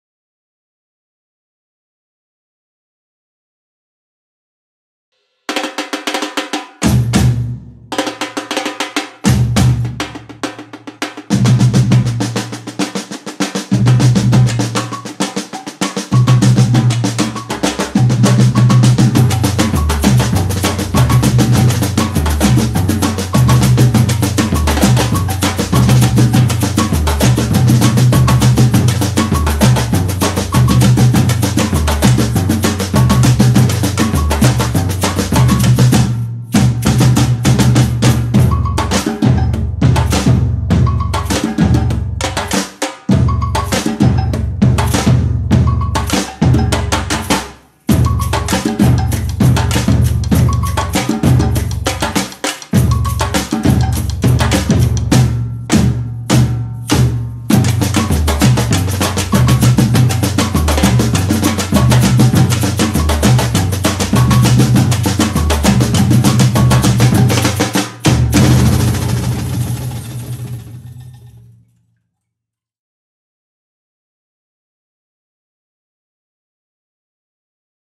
Percussion Workshops Appetizer 2°
all instruments played by Maxim Zettel
recorded and edited by Daniel Hahnfeld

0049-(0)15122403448